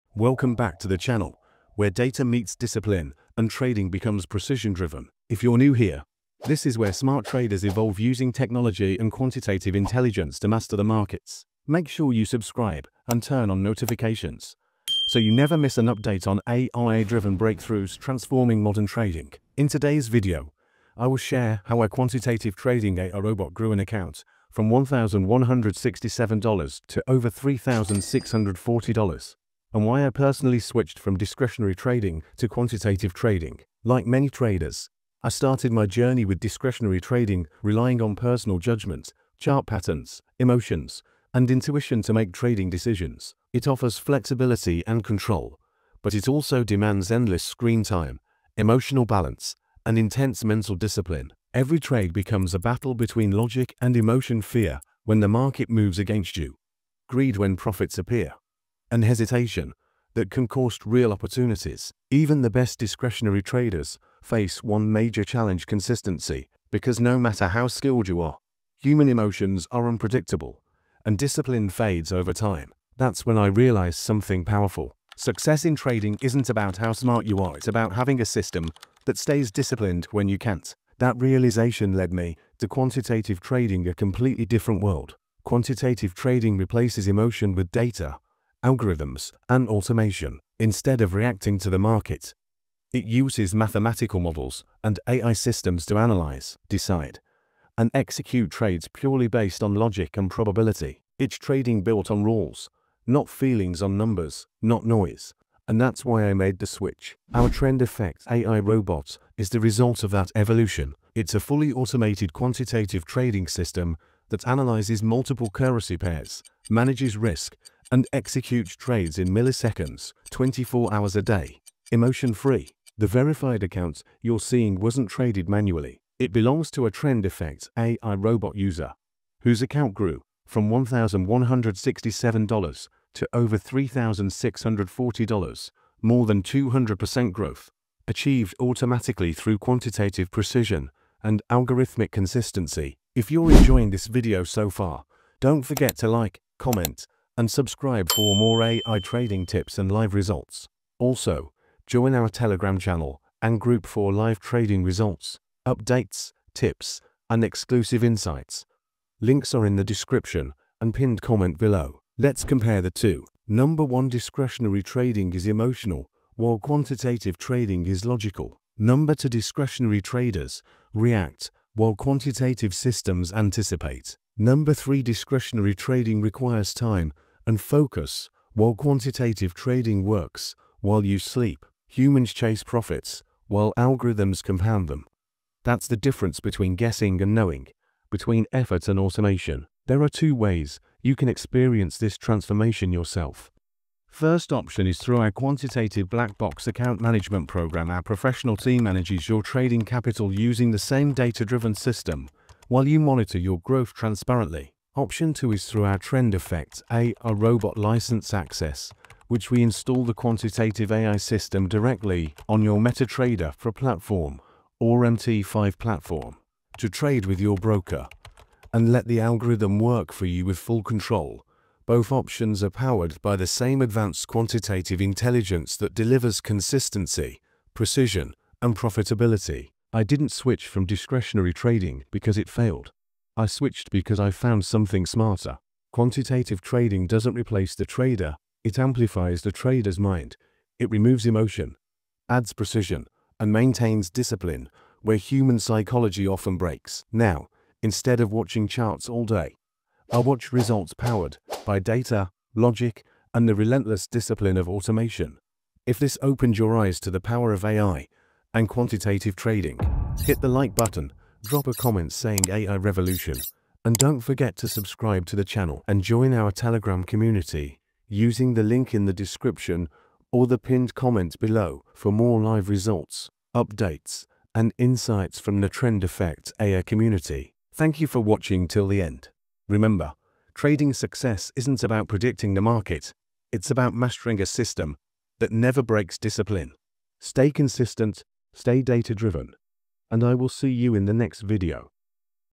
Quantitative Trading vs Discretionary Trading| How a Quant Trading Bot Turned $1,167 into $3,648
Are you curious about the real difference between quantitative trading and discretionary trading? In this video, we break down both trading styles, showing how data-driven systems outperform emotional decision-making in today’s fast-moving financial markets.

Get Access to the Quant Trend Effect AI Robot

💡 Join hundreds of traders already using AI technology to build daily passive income from Forex.

You will  also see a practical example of how our Quant Trading Bot transformed a $1,167 account into $3,648 demonstrating the power of algorithmic precision, mathematical models, and automated execution.

🔍 What You will Learn in This Video:
• The key differences between Quant Trading and Discretionary Trading

• Why quantitative systems offer consistency, speed, and emotion-free decision-making

• How algorithmic models spot opportunities the human eye may miss

• Live breakdown of how the bot achieved a 3× account growth

• The future of AI and automation in Forex & financial markets

🤖 Why Quantitative Trading?
Quant trading relies on tested statistical models, backtested strategies, and automation helping traders avoid fear, greed, and hesitation that often affect discretionary traders.

💡 Thinking of Switching to Quant Trading?
This video provides clarity on why more traders are moving from manual trading to algorithmic systems for long-term consistency and growth.

0:00 – 0:30 — Introduction
0:31– 0:45 — My Background in Discretionary Trading
1:13 – 1:25 — Why I Started Exploring Quantitative Trading
1:26 – 2:42 — What Is Quantitative Trading?

2:06 – 2:23— How the Quant Bot Turned $1,167 into $3,648

2:43 – 3:02 — Quant Trading vs Discretionary Trading

3:03 – 3:14 — Why I Fully Switched to Quantitative Trading

3:15 – 4:53 — How to Get Started